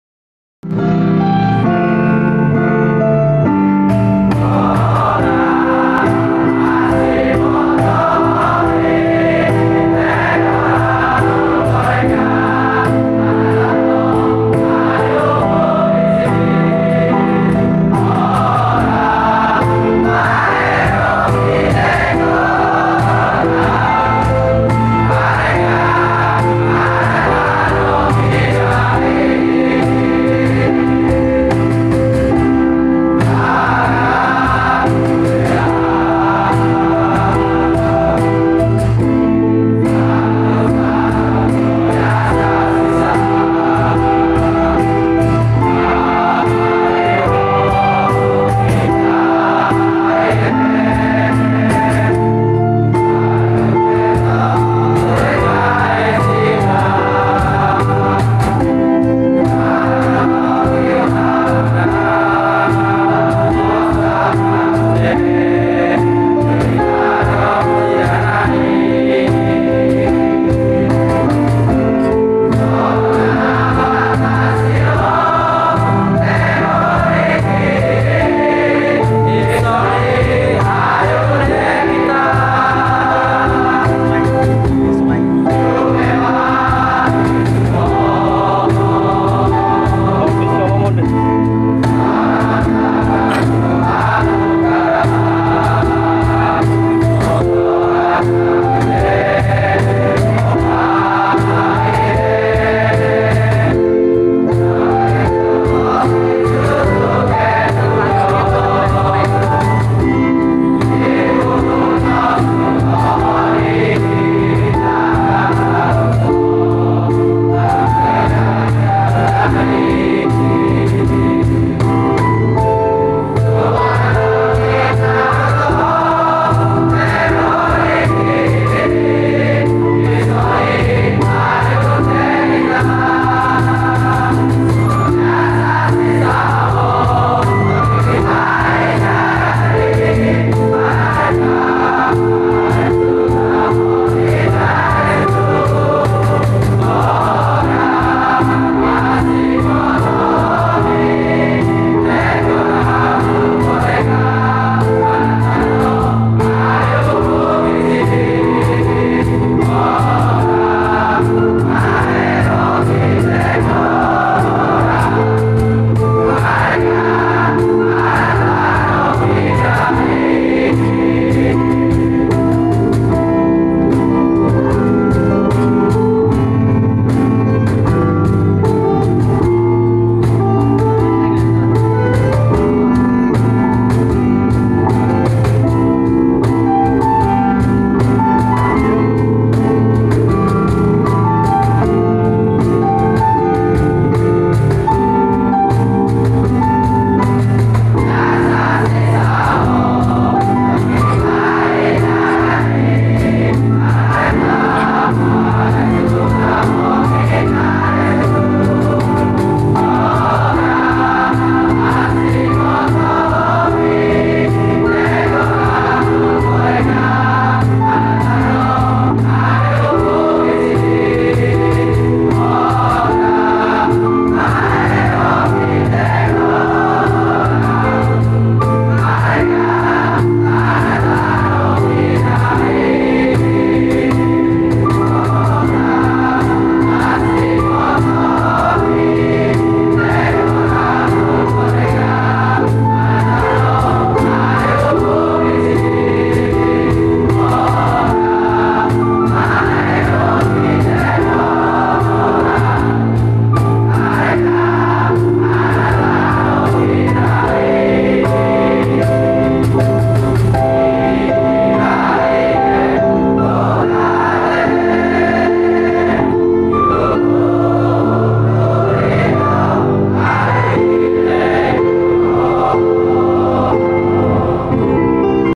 Penutupan Pelatihan Daerah Angkatan 312 Pemagangan Ke Jepang
Gladi resik penutupan pelatihan daerah program magang ke Jepang kerjasama Kemnaker RI dengan IM Japan.
angkatan 312 di Technopark Sragen Jawa tengah.